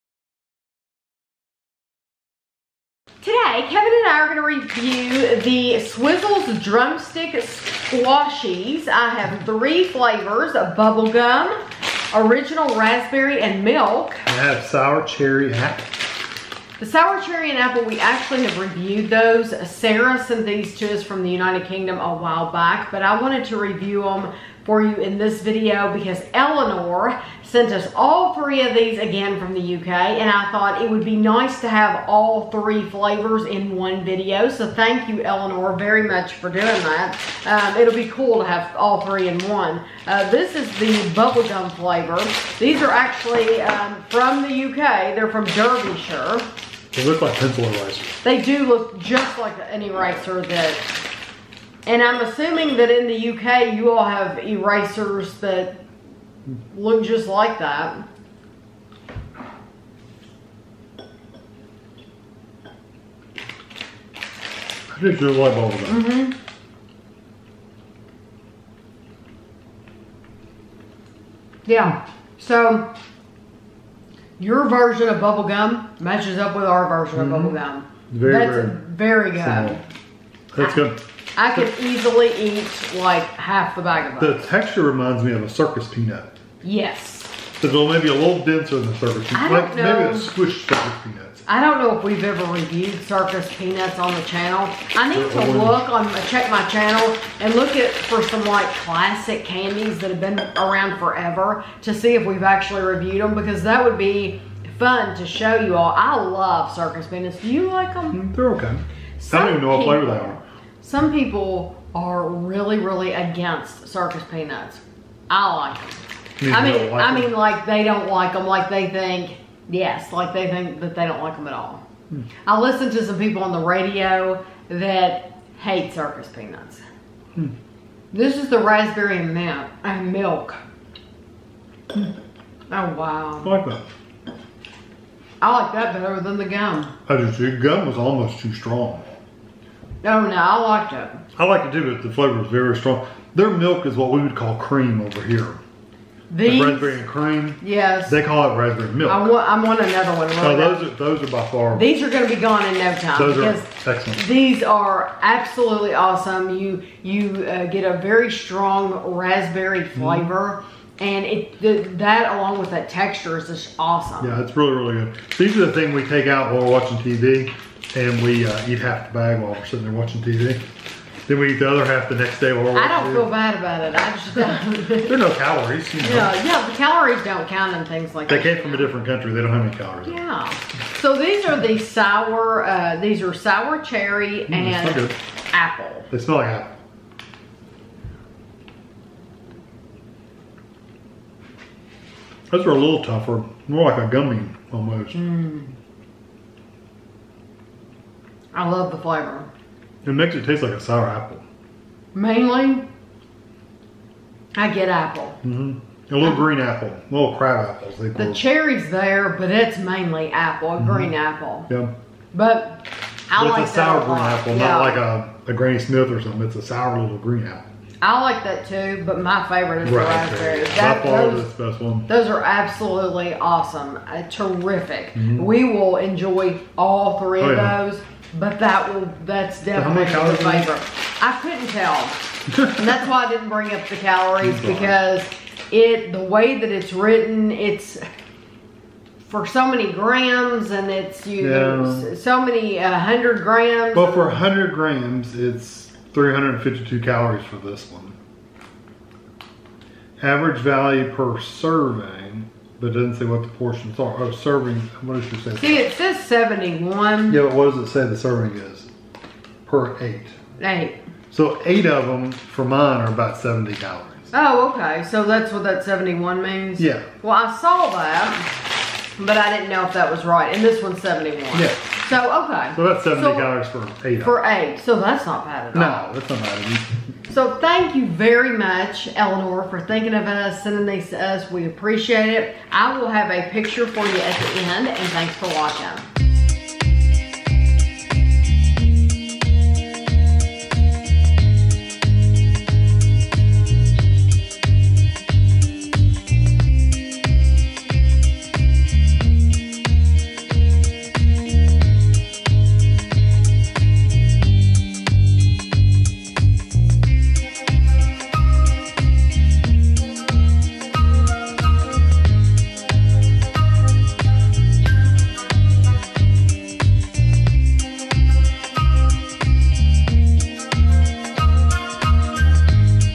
Swizzels Drumsticks Squashies: Bubblegum, Original Raspberry & Milk, Sour Cherry & Apple Review
This is a taste test/review of the Swizzels Drumsticks Squashies in three flavours including Bubblegum, Original Raspberry & Milk and Sour Cherry & Apple. They were mailed to us from Eleanor in the UK!
* Original Raspberry & Milk
* Sour Cherry & Apple